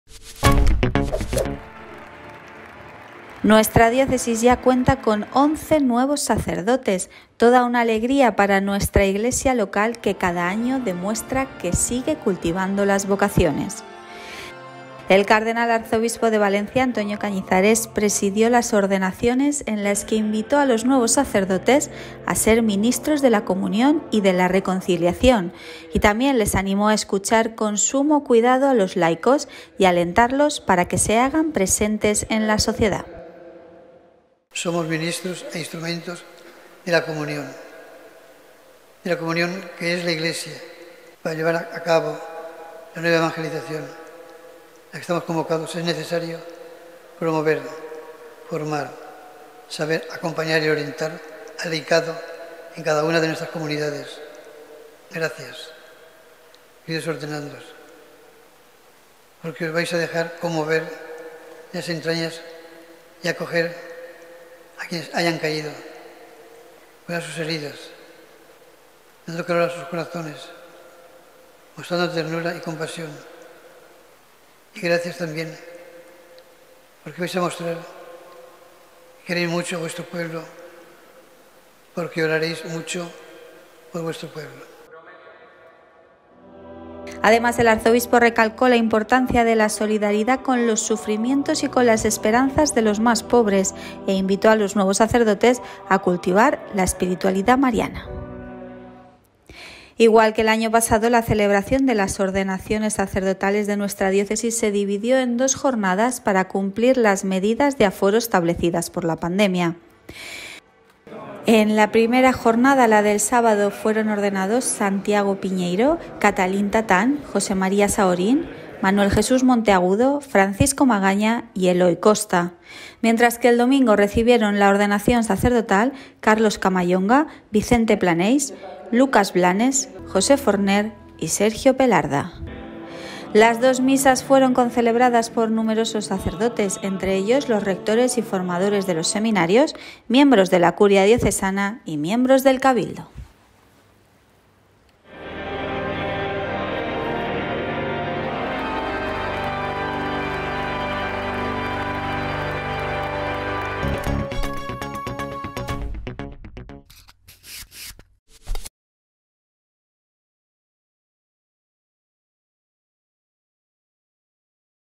Ordenaciones sacerdotales 2021 en la Catedral de Valencia
El cardenal arzobispo de Valencia, Antonio Cañizares, ha invitado a los once nuevos sacerdotes ordenados este fin de semana en la Catedral a ser “ministros de la comunión y la reconciliación” y a “escuchar con sumo cuidado a los laicos, valorar su experiencia, recoger sus iniciativas, guiarles con fe eclesial, atender sus sugerencias, fomentar su participación y alentarles para que se hagan presentes en esta sociedad, como discípulos de Cristo, para recomponer el tejido social”.